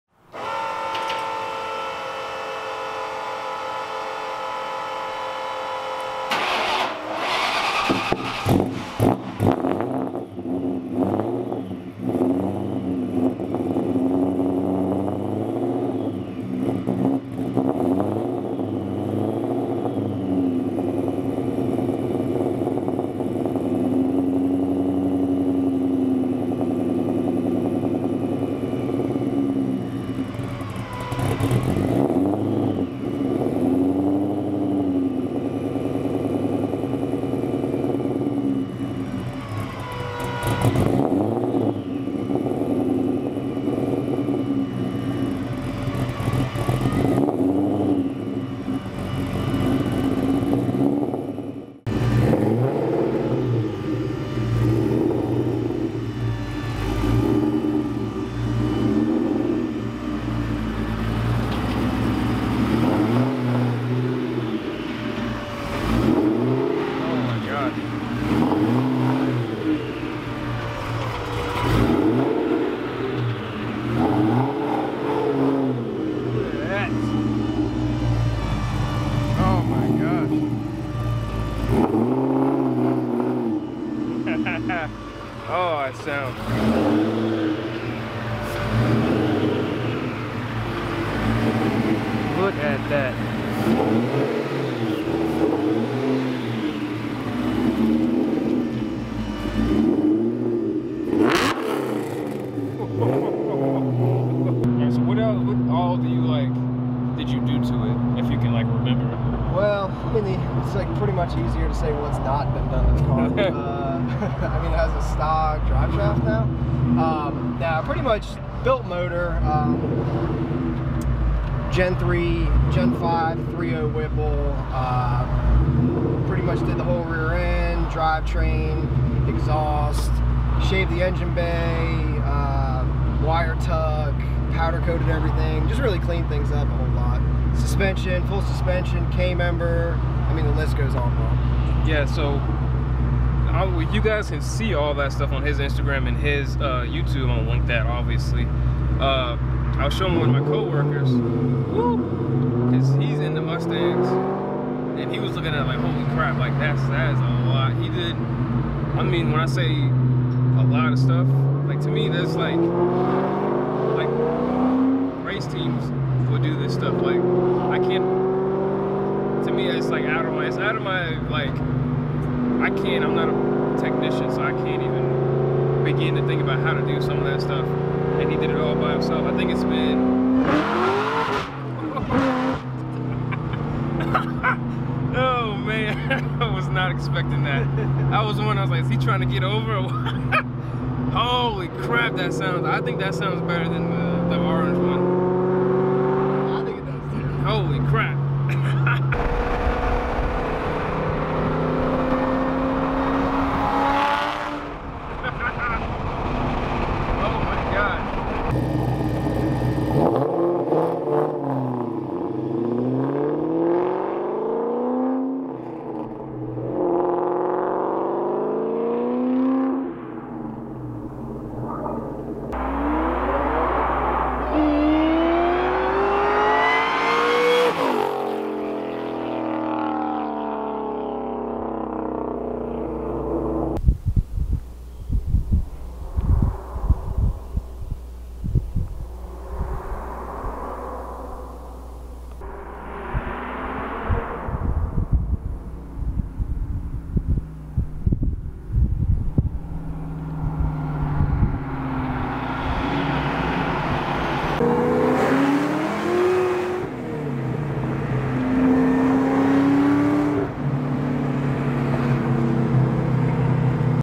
Terminator Cobra Ride Along
Ride along video filmed and edited by @LamboDEB1 in my 850HP 2003 Cobra.